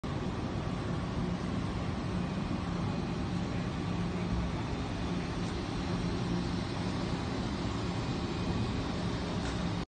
Tamila Holub e José Paulo Lopes já treinam em Barcelona
José Paulo Lopes e Tamila Holub, nadadores do SC Braga que se encontram ao serviço da Seleção Nacional de Natação, já se encontram em Barcelona para participar na terceira etapa do Circuito Mare Nostrum.